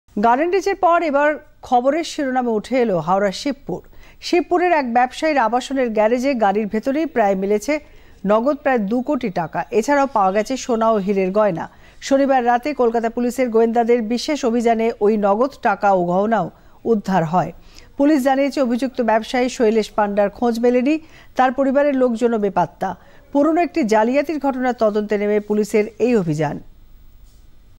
হাওড়ায় ব্যবসায়ীর গাড়িতে উদ্ধার নগদ ২ কোটি টাকা
হাওড়ার শিবপুরের ব্যবসায়ী শৈলেশ পাণ্ডার আবাসনে কলকাতা পুলিশের গোয়েন্দা বিভাগের বিশেষ অভিযান, গ্যারেজে গাড়ির ভিতরে নগদ প্রায় ২ কোটি টাকার সাথে উদ্ধার সোনা ও হীরের গয়না।